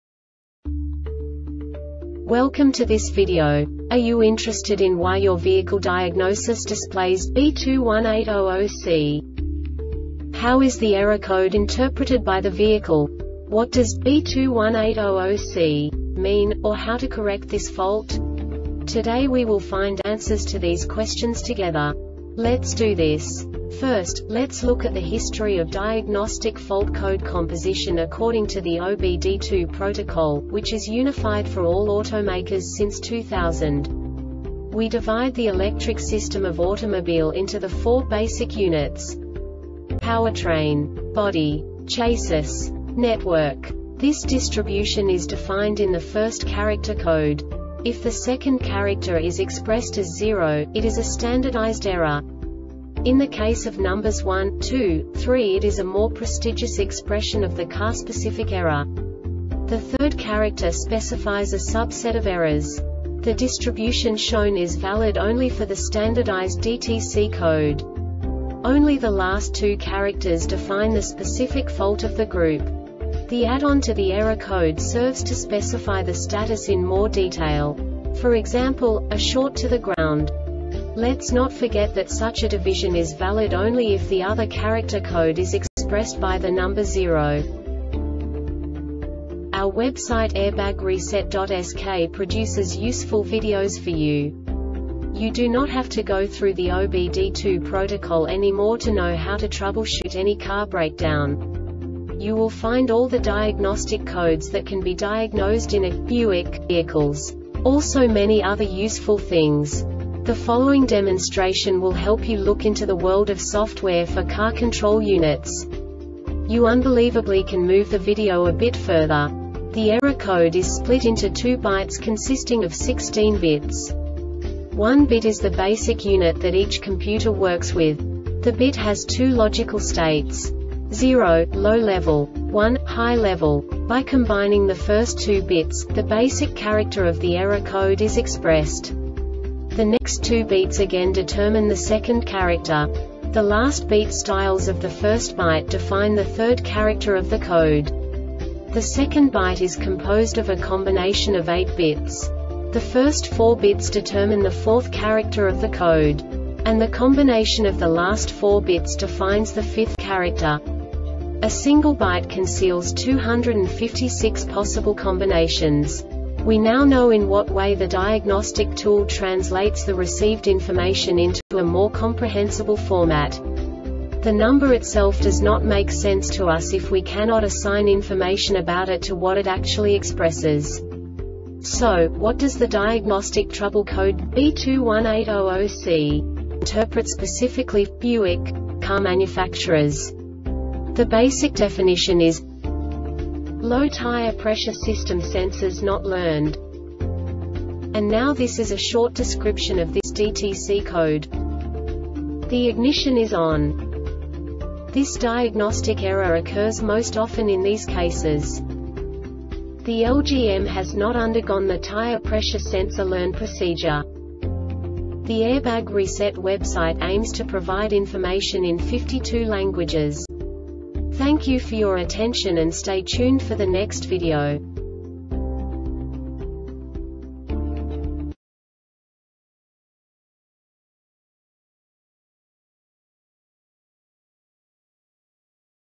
DTC Buick B2180-0C Short Explanation
Contents:
0:21 Basic DTC analysis according to OBD2 protocol standard.
1:48 Insight into programming
2:58 A diagnostically understandable description of the Diagnostic Trouble Code
3:05 Basic error definition

Make:
Buick
Code:
B2180 0C
Definition:
Low Tire Pressure System Sensors Not Learned
Description:
The ignition is ON.
Cause:
The LGM has not undergone the tire pressure sensor learn procedure.